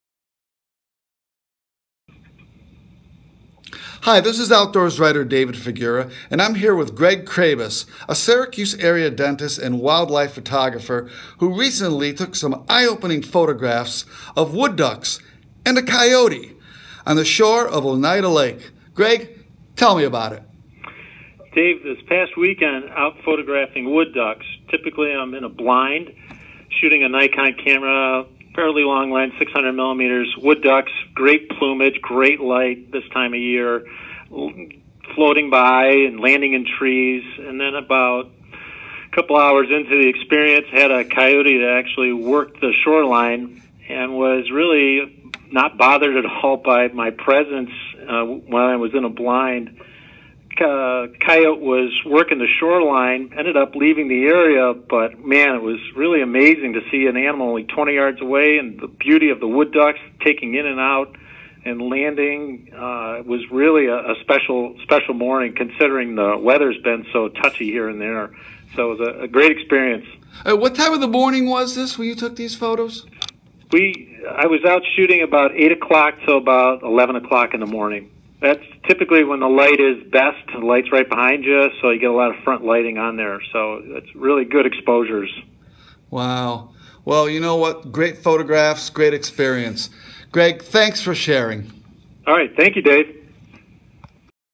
CNY wildlife photographer captures wood ducks and coyote on film
Greg Craybas, a Syracuse area dentist photographer, took some eye-opening photos of wood ducks and a coyote from a blind on the shore of Oneida Lake.